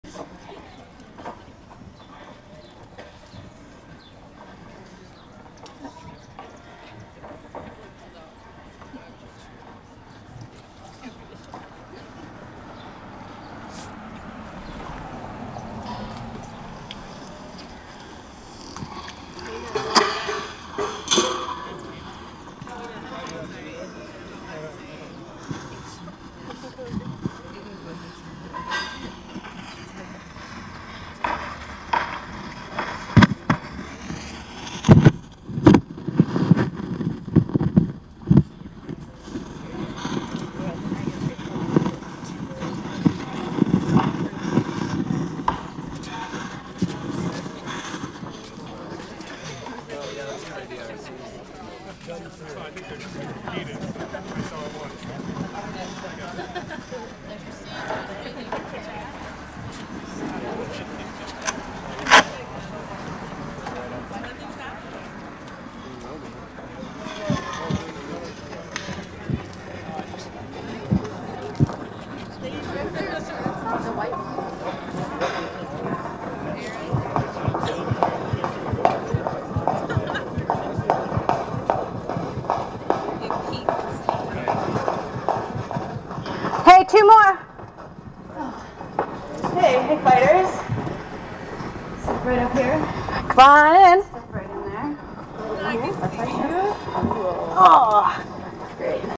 Hey Fighter
Gallery Night 2011 this is a performance piece in which two women were taking up boxing one another.